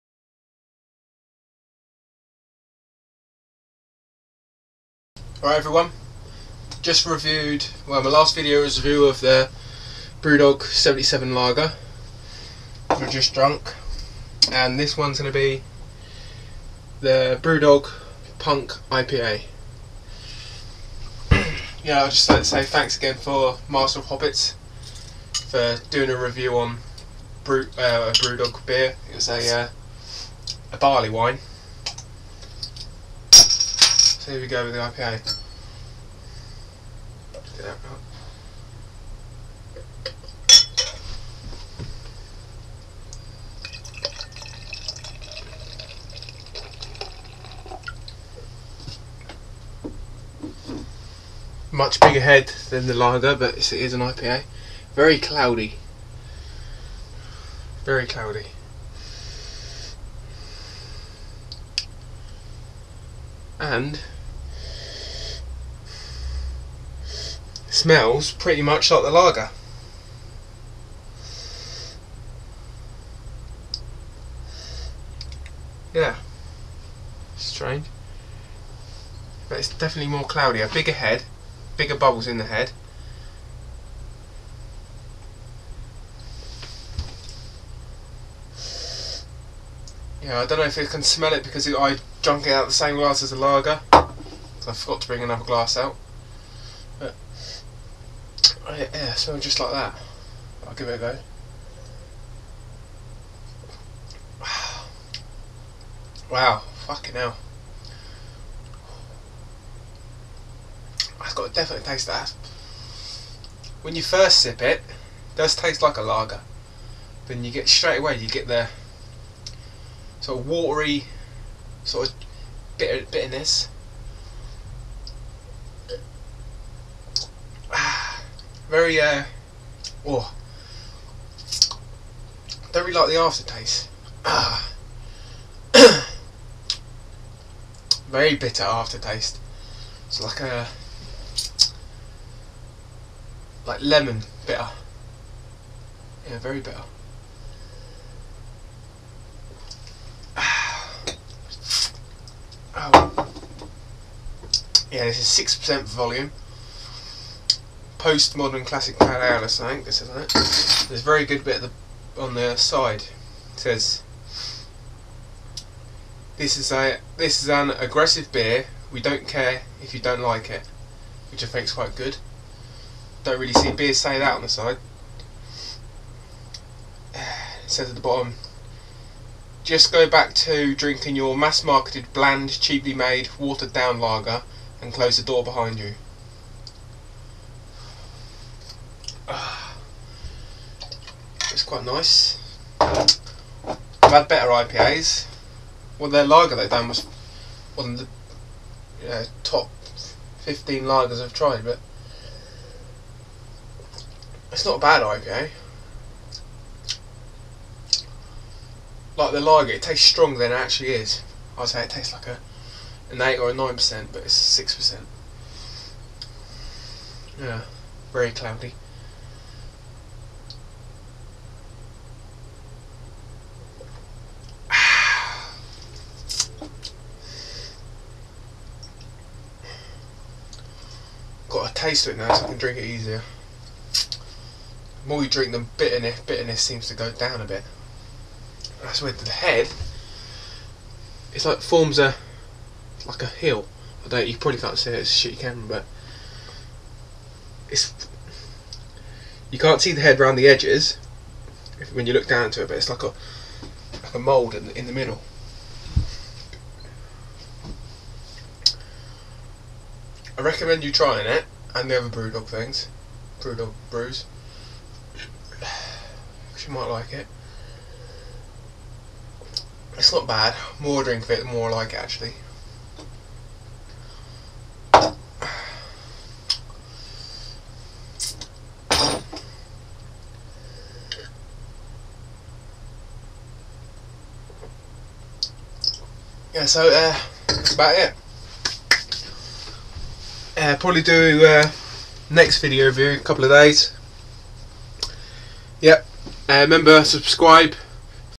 BEER REVIEW 4: BREW DOG PUNK IPA
BEER REVIEW 3: BREW DOG PUNK IPA
very good ipa, defiantly worth a go, as well as the lager

that is a very good website, they deliver all over the place.

the master of hoppets channel great beer reviewer

next video up on wednesday (dont know what i will be reviewing yet all suggestions welcome looking for a nice ipa or lager)